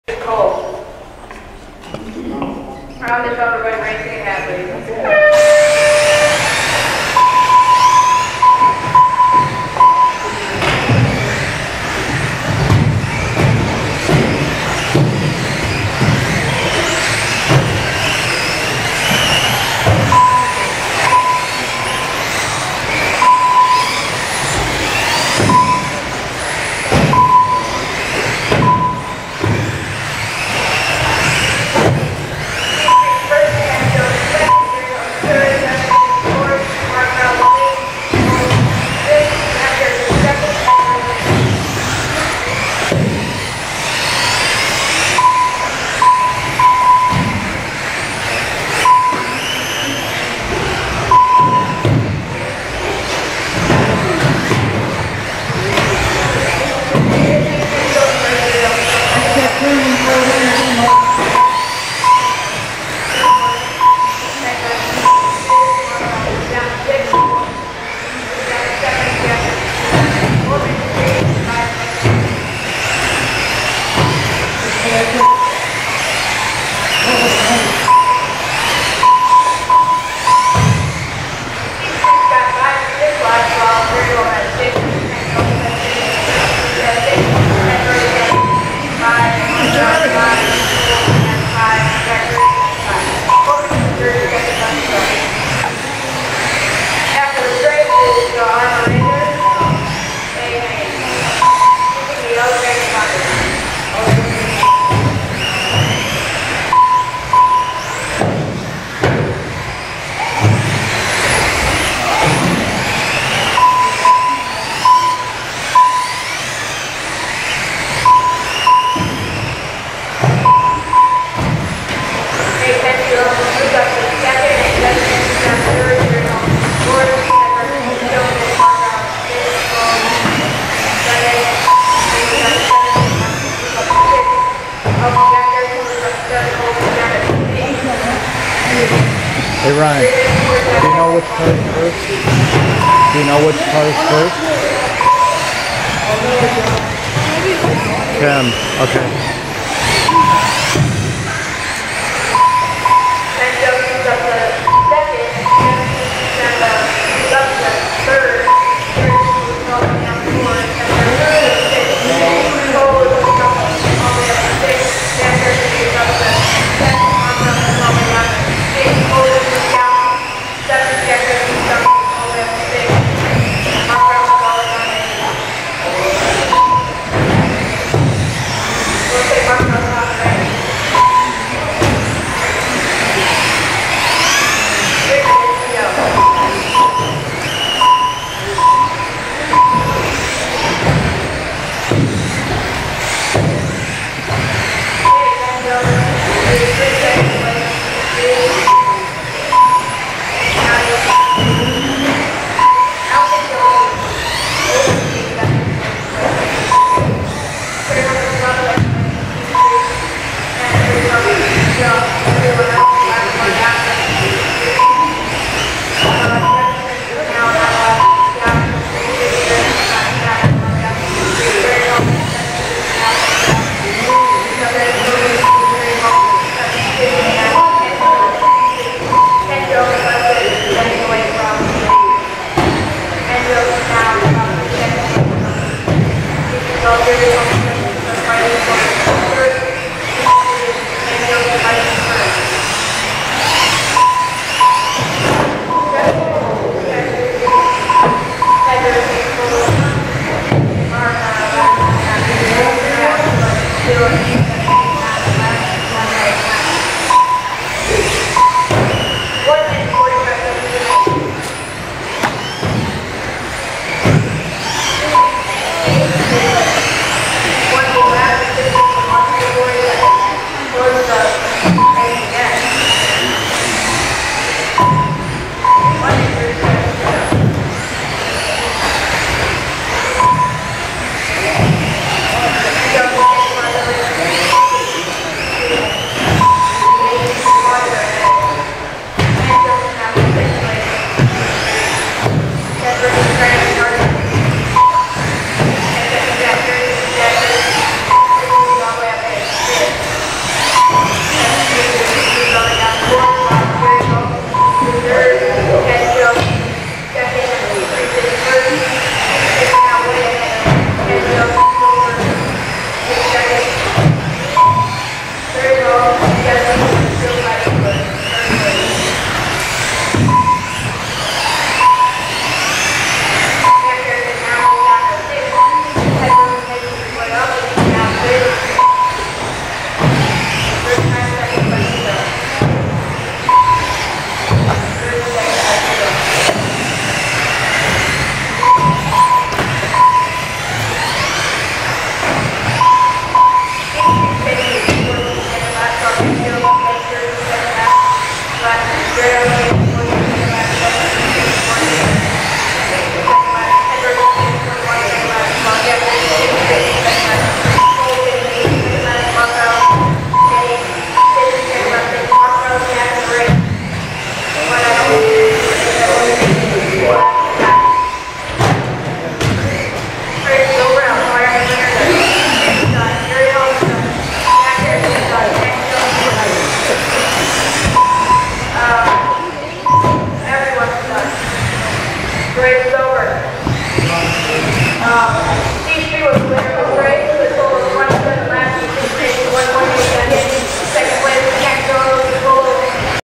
Hadley RC 2016 Invitational Adult Off Road A Main
Hadley RC Club, 2016 Invitational Race, Adult Off Road - A Main